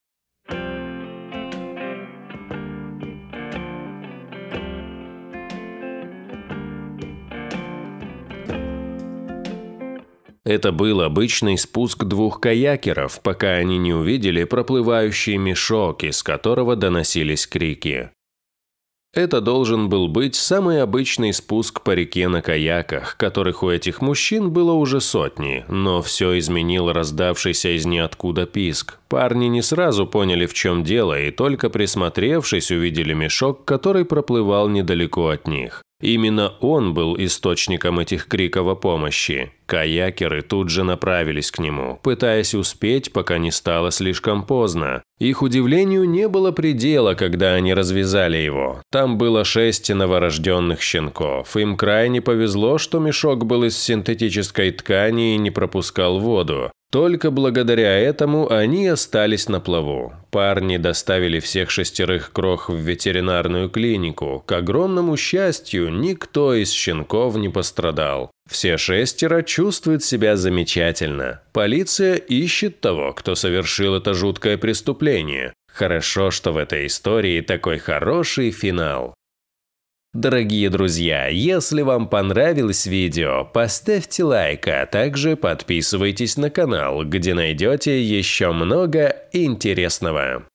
Люди увидели мешок плывущий по реке, из которого доносился писк, содержимое мешка может шокировать
Это должен был быть самый обычный спуск по реке на каяках, которых у этих мужчин было уже сотни. Но все изменил раздававшийся из ниоткуда писк. Парни не сразу поняли в чем дело и только присмотревшись, увидели мешок, который проплывал недалеко от них. Именно он был источником этих криков о помощи.